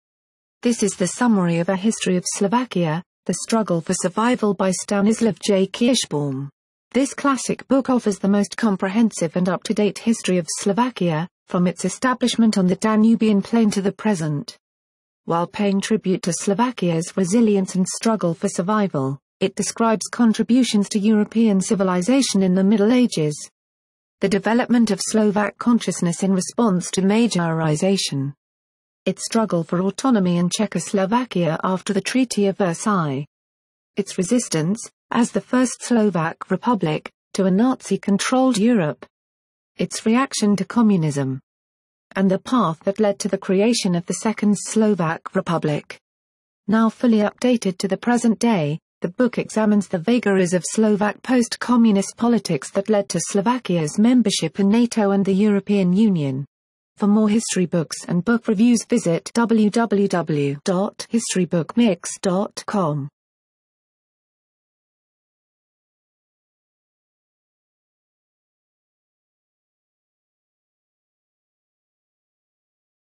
History Book Review: A History of Slovakia: The Struggle for Survival by Stanislav J. Kirschbaum
This is the summary of A History of Slovakia: The Struggle for Survival by Stanislav J. Kirschbaum.